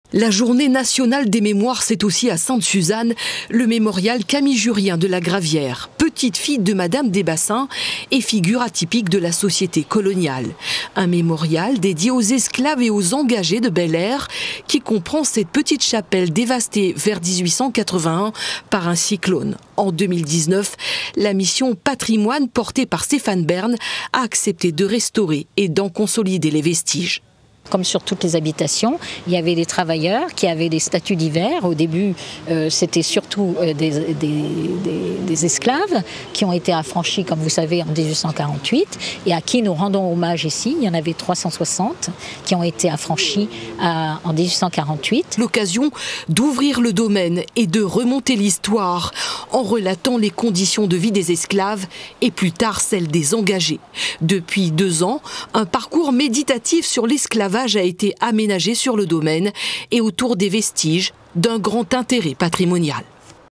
Commémoration de la journée nationale des mémoires de la traite négrière
10 mai : Sainte-Suzanne commémore la journée nationale des mémoires de la traite négrière, de l’esclave et de leur abolition.